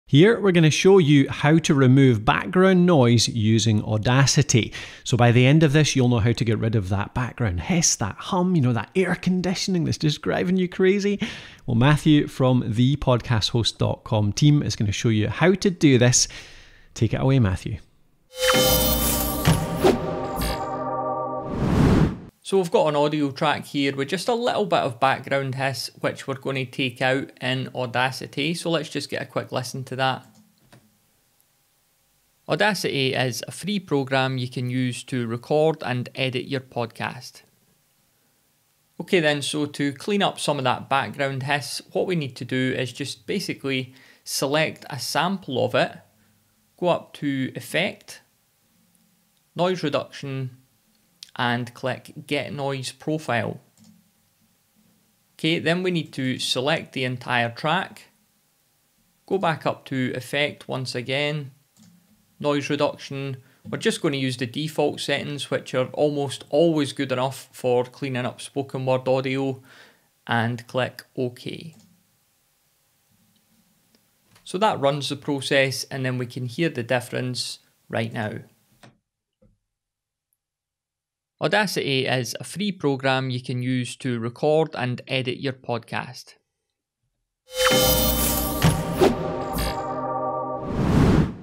Noise Reduction in Audacity: How to reduce hiss, hum & other background sounds
Here's how to use the Audacity noise reduction feature, to reduce background noises, such as hiss and hum. You'll often get this from a computer in the background, or air conditioning, or other regular sounds.